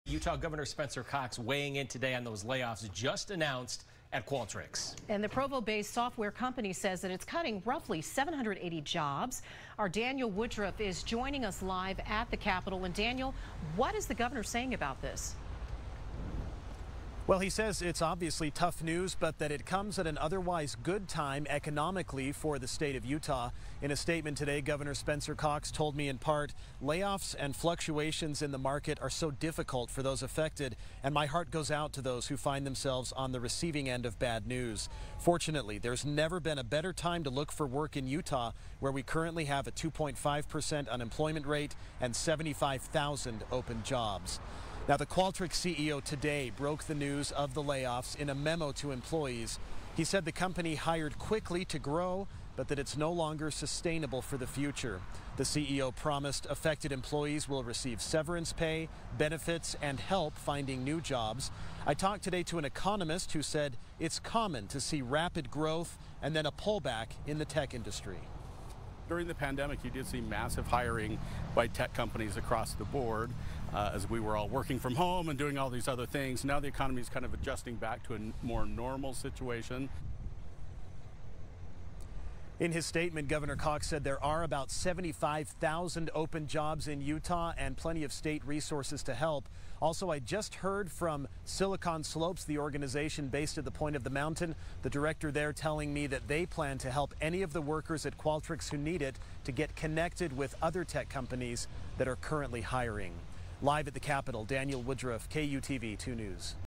Utah Gov. Spencer Cox responds to Qualtrics laying off nearly 800 people
Qualtrics, a Utah-based business software company, has announced plans to eliminate approximately 780 roles across the company.

Gov. Spencer Cox said it's tough news but that it comes at an otherwise good time economically for the state of Utah.